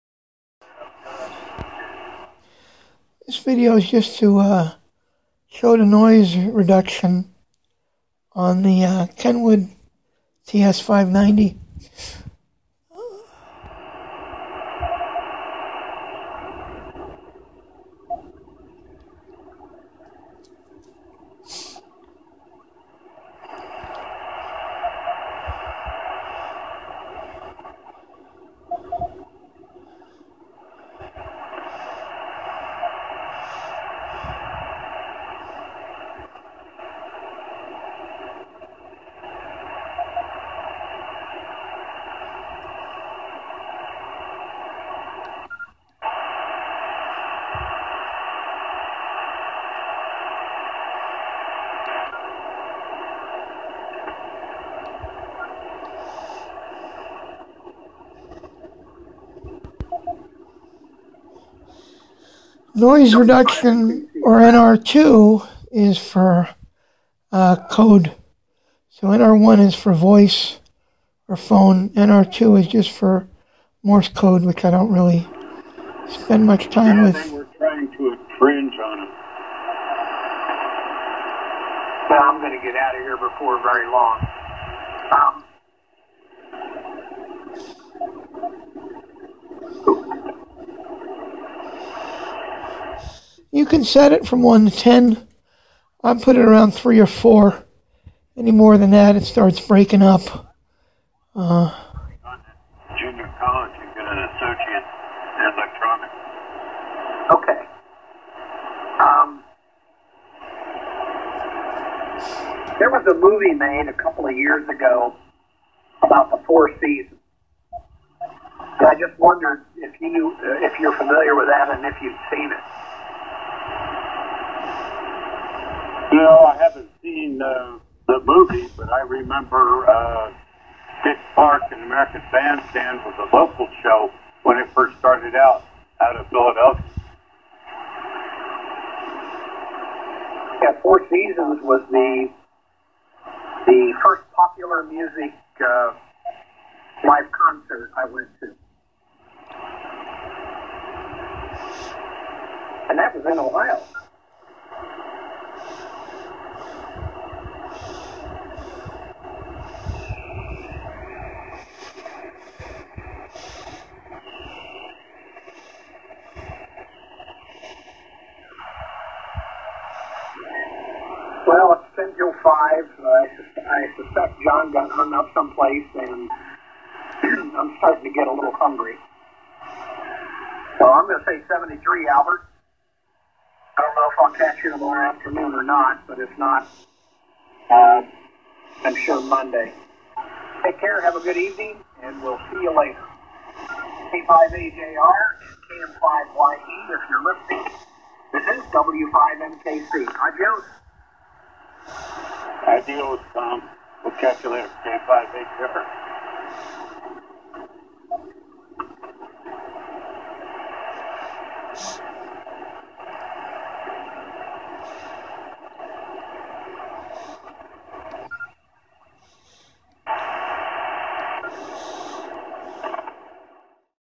Kenwood ts-590 noise reduction
Demonstrating the amazing DSP noise reduction on the Kenwood ts -590